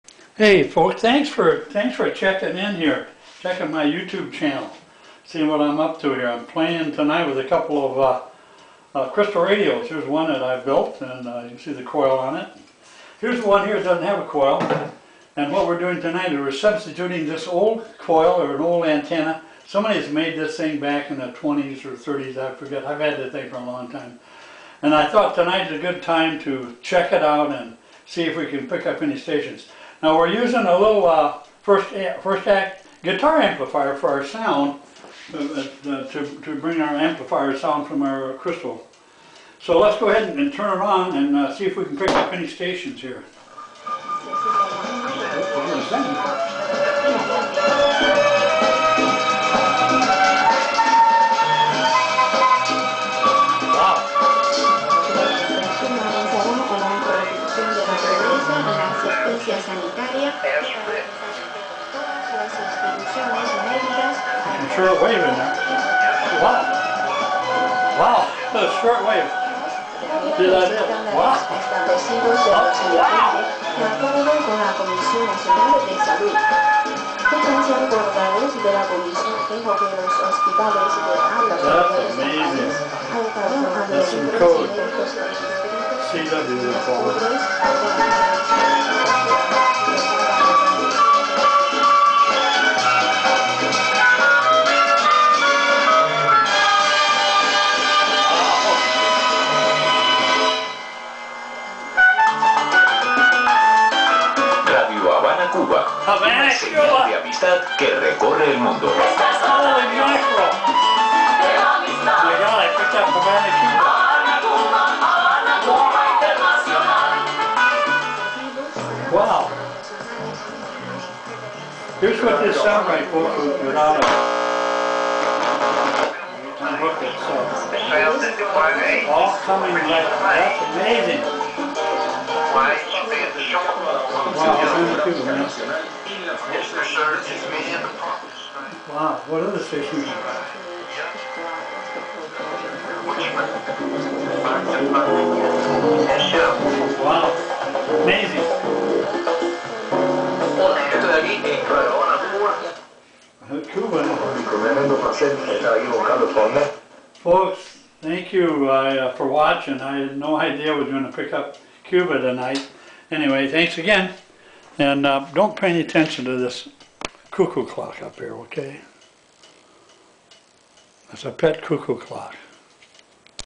S.W Radio with a Crystal set....Antique coil bring in shortwave no Foxhole
Shocked to get Havana Cuba ..I am about 50 miles south of Ft.Worth
Texas... pay no attention to my pet coco clock